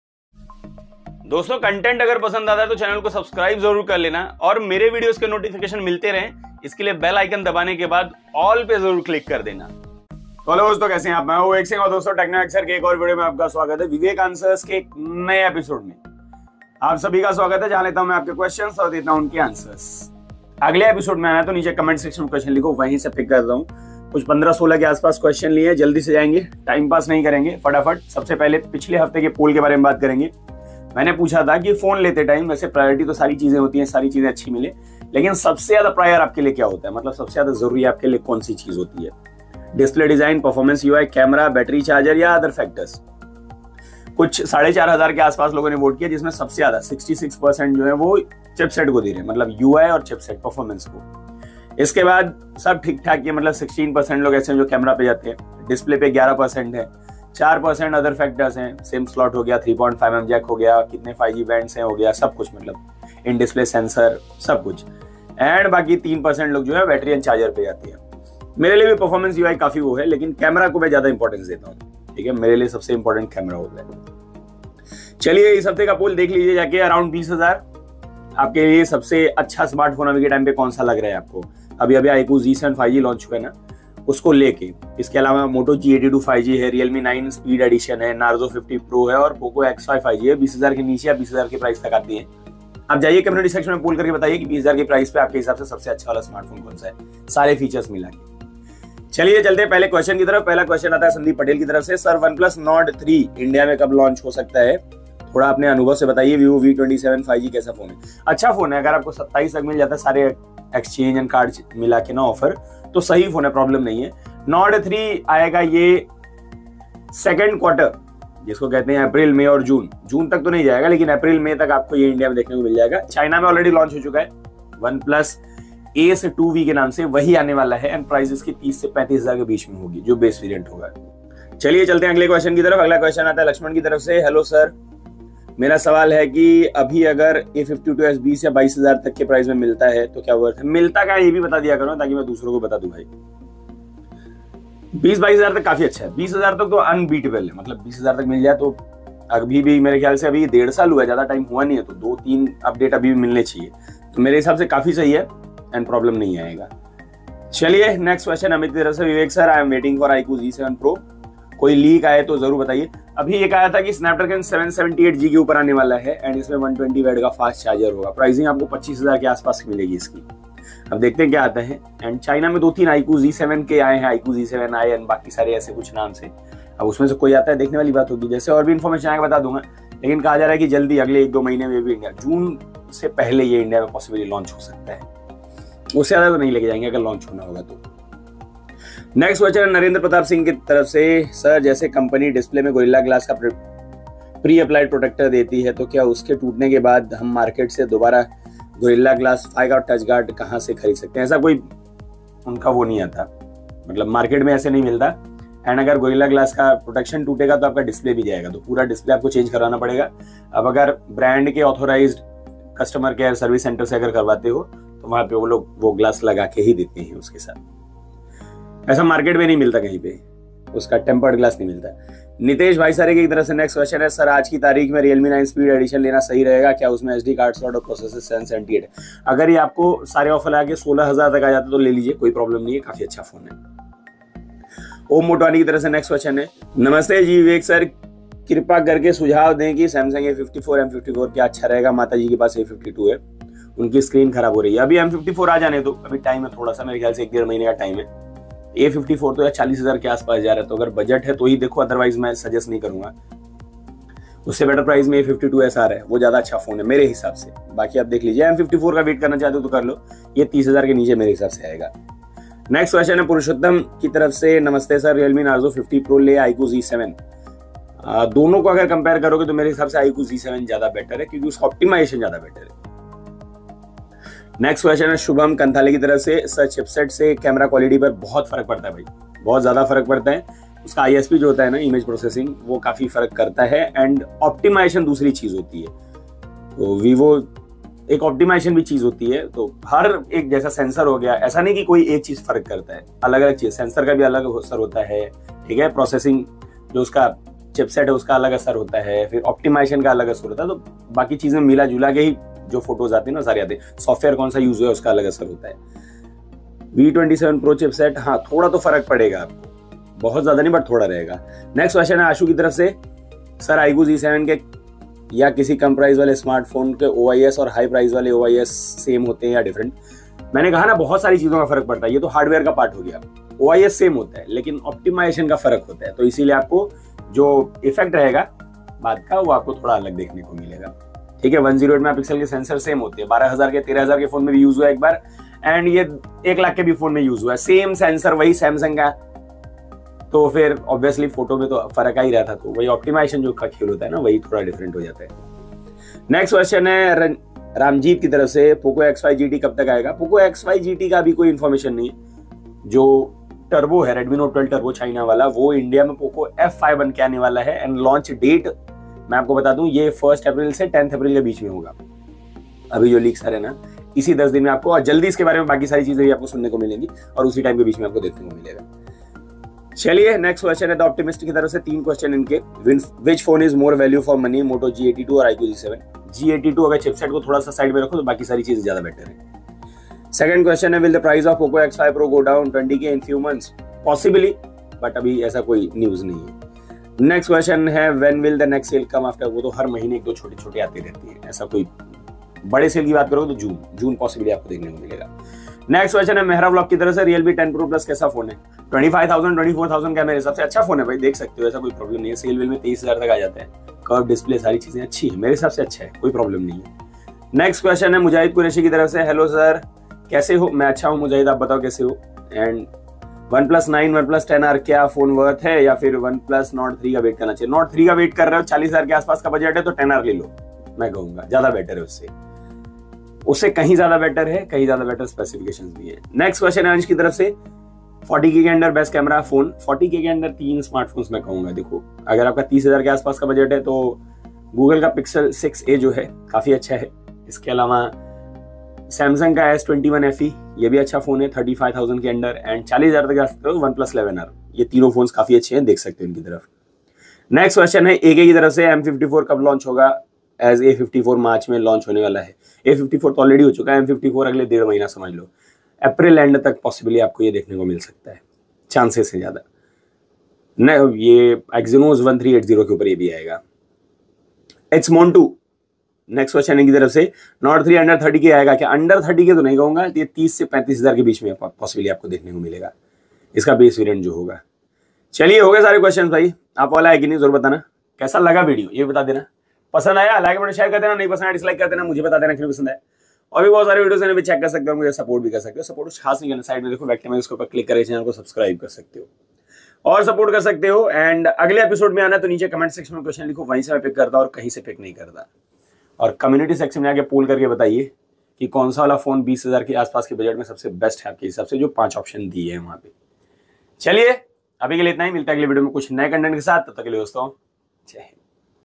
IQOO Z7 PRO 120w INDIA, SAMSUNG M54 5G, NORD 3 INDIA,POCO F5 LAUNCH, SAMSUNG A52S@20K
नमस्ते ‌दोस्तों, TECHNO VEXER के नए विडीयों में आपका स्वागत है, दोस्तों VivekAns का 137वां Episode देखना है तो ये विडीयों अंत तक देखें

VIDEO HIGHLIGHTS:

00:00 Introduction
00:28 Poll Result
01:13 New Poll
01:38 Nord 3 India
02:10 Samsung A52s @20k
02:37 Iqoo Z7 Pro India
03:08 Gorilla Glass Tempered
03:41 Realme 9 Speed Worth Hai ?
03:54 Samsung M54 vs A54
04:22 Iqoo Z7 vs Narzo 50 Pro
04:34 Importance of Chipset
05:11 Cheaper OIS vs Flagship OIS
05:46 Poco X5 GT (Poco F5)
06:09 Moto G82 vs Iqoo 7
06:35 Realme 10 Pro plus ?
06:46 Oneplus Nord 3 vs 10R
07:06 Best Camera phone Under 40k
07:26 Samsung M54 India
07:44 Nord 3 Price ?
07:54 Conclusion

Friends You also  can follow me on:
दोस्तों यहां आप मुझे फाॅलो भी कर सकते हैं:

धन्यवाद